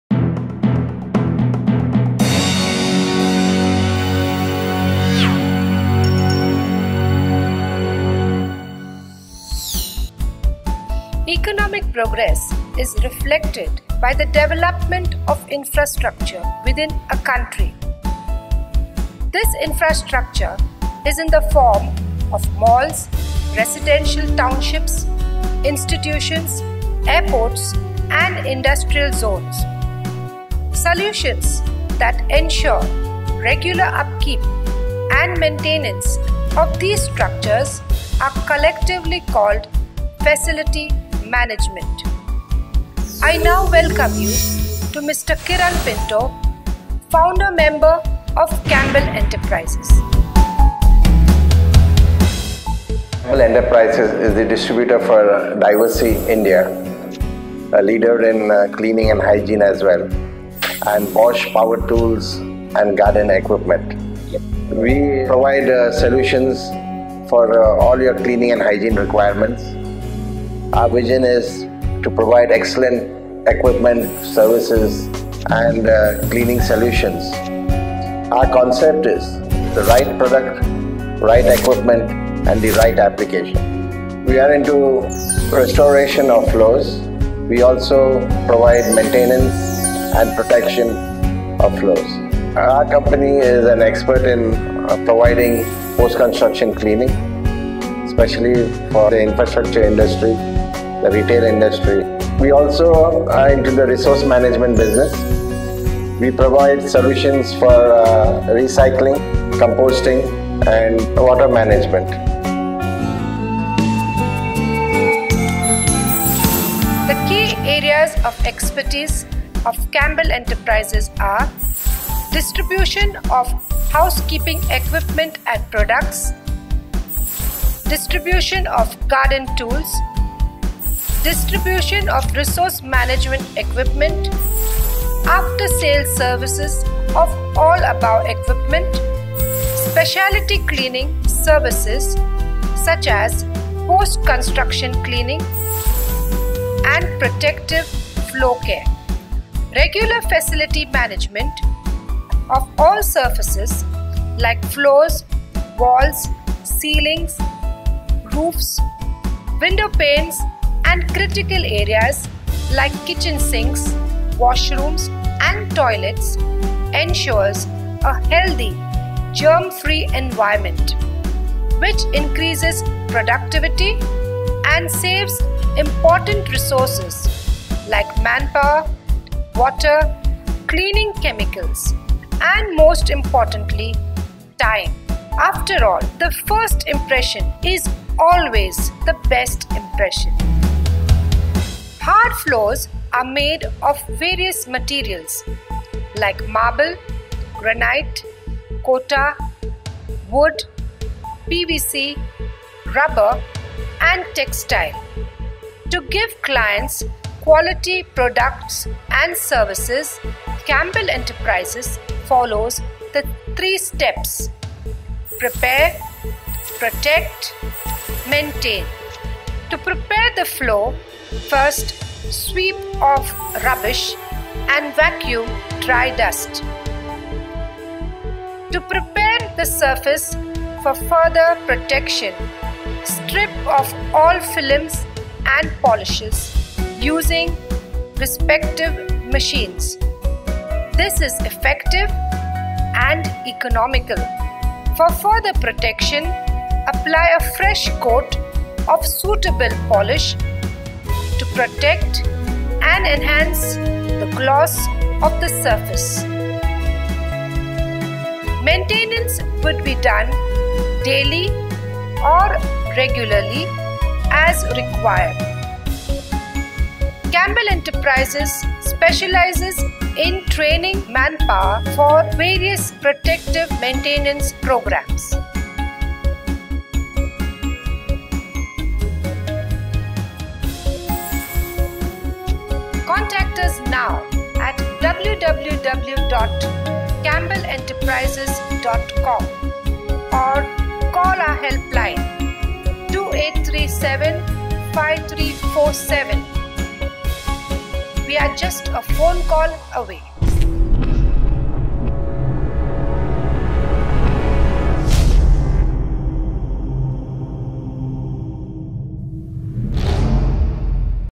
Cambell Enterprises
Cambell Enterprises is a company engaged in cleaning and hygiene solutions and is in operation for over 20 years. Cambell Enterprises is the distributors for Diversey India and Bosch Garden Equiptments.